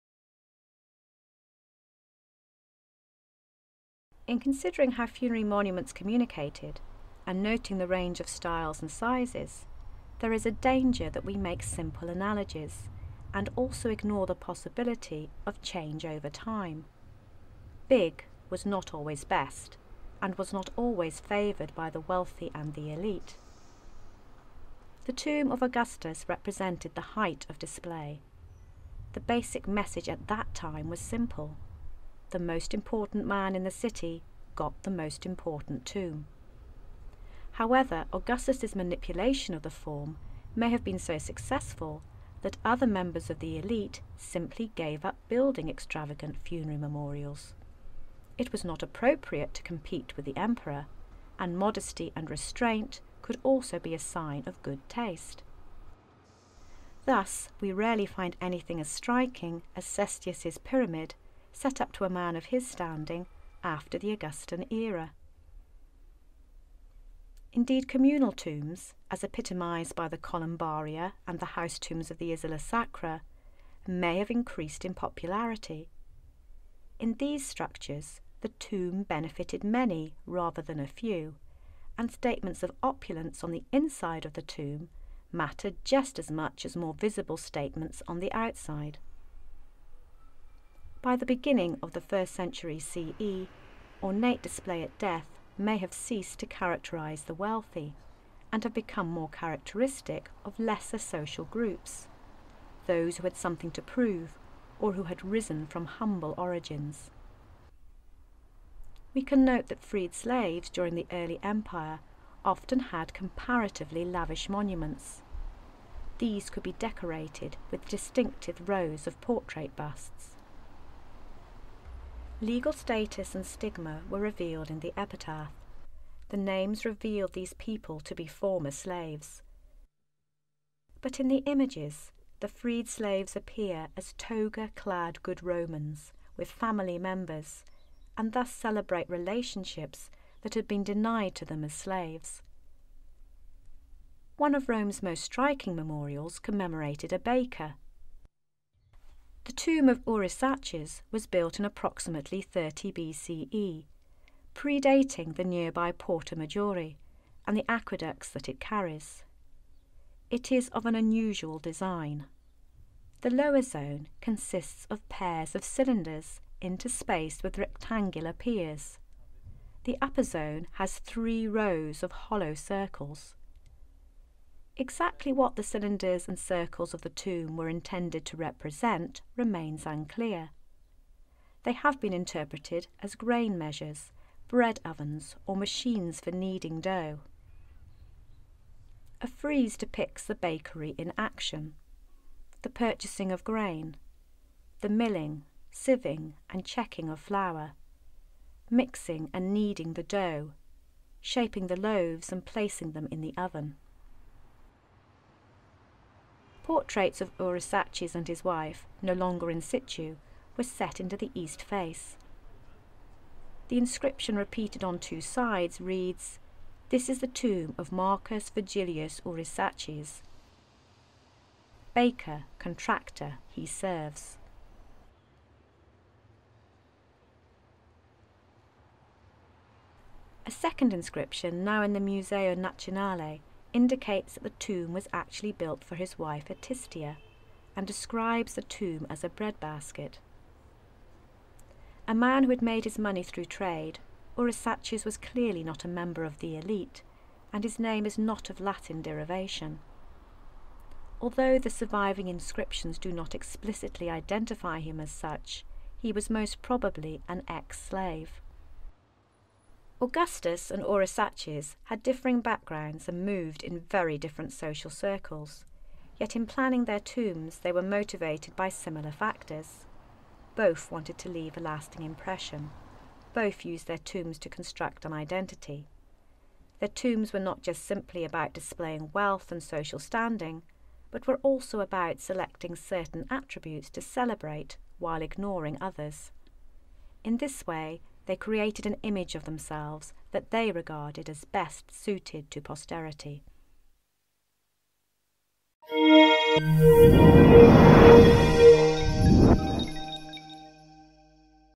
Constructing Identity - Roman Funerary Monuments (5/7)
How tomb styles changed after Augustus built his huge mausoleum.